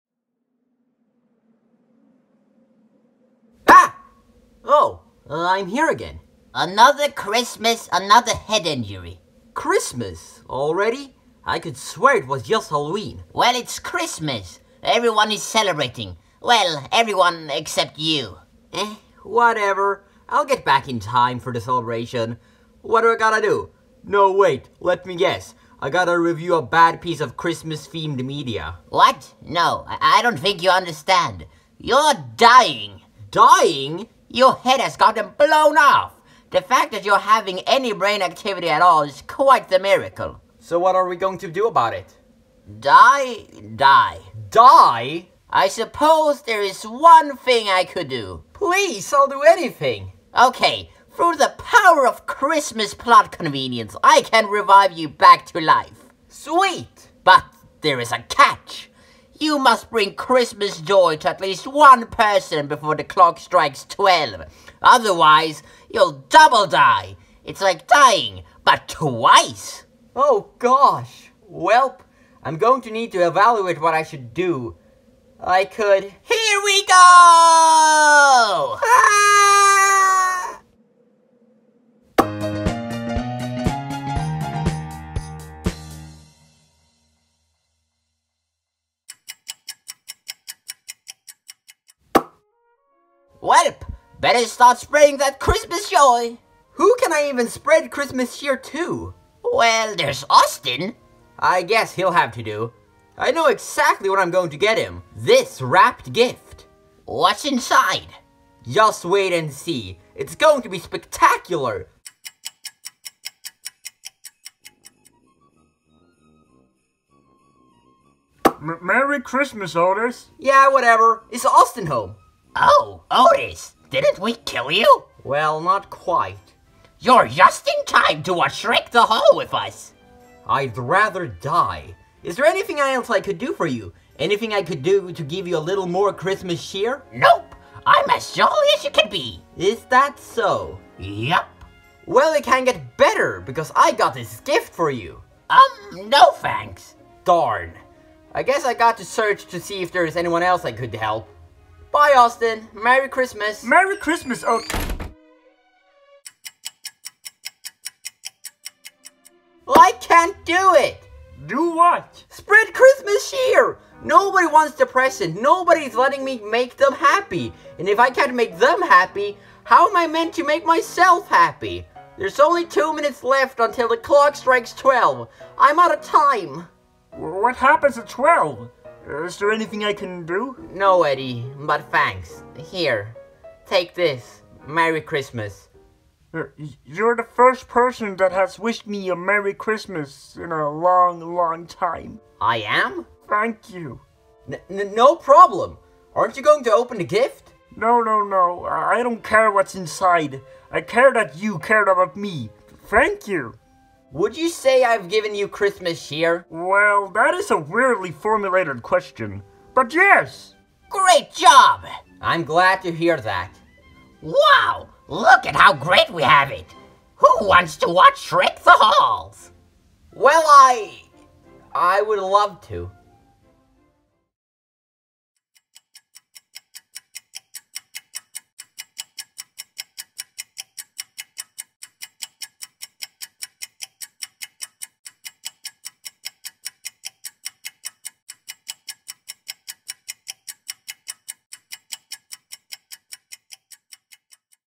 A Very Shootin N Pootin Christmas
As a gift I bring you a "big" Christmas special about the true meaning of the holidays, treating your friends and family with the respect that they and you deserve.
Happy holidays!